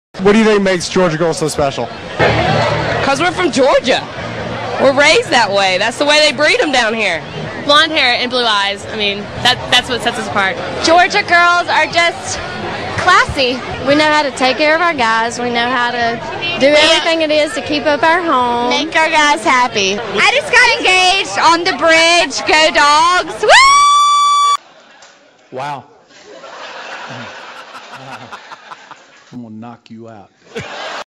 Paul Johnson's Thoughts on Georgia Girls
Georgia Tech Head Coach Paul Johnson offers his thoughts on UGA girls.